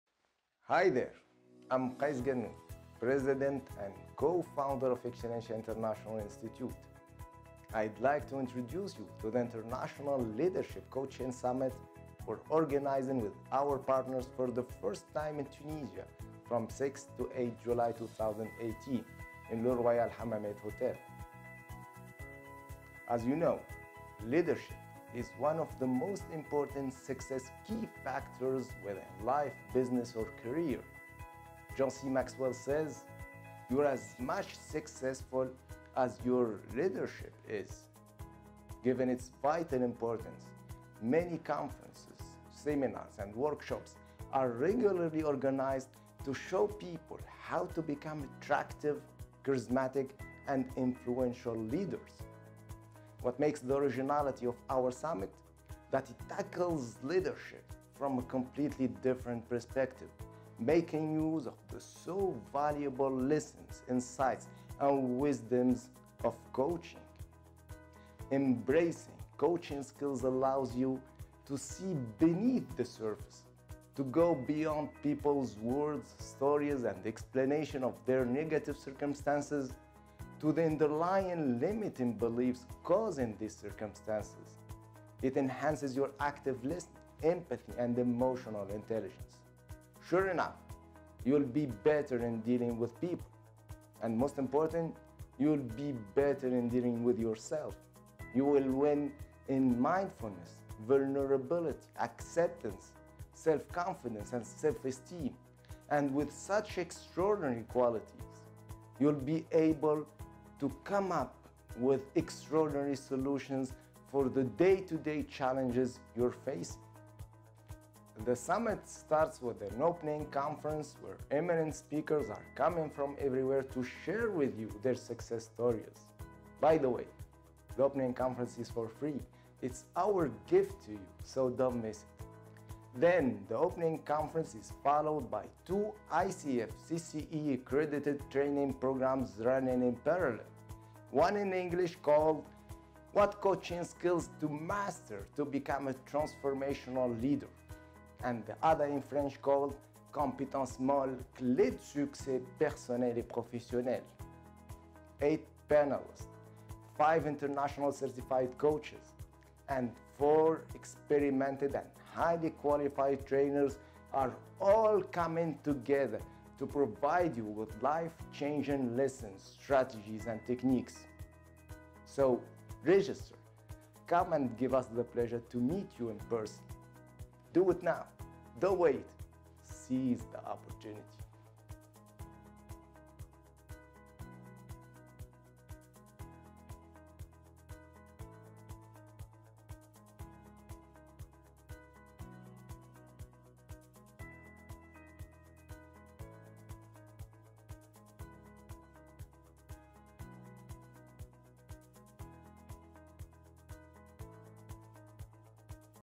Invitation to the International Leadership Coaching Summit
I am pleased to invite you to the International Leadership Coaching Summit we are organizing with our partners from 6 to 8 July 2018 in Le Royal Hammamet Hotel. This is an introductory video highlighting the originality of the Summit and the value-added it brings to the table.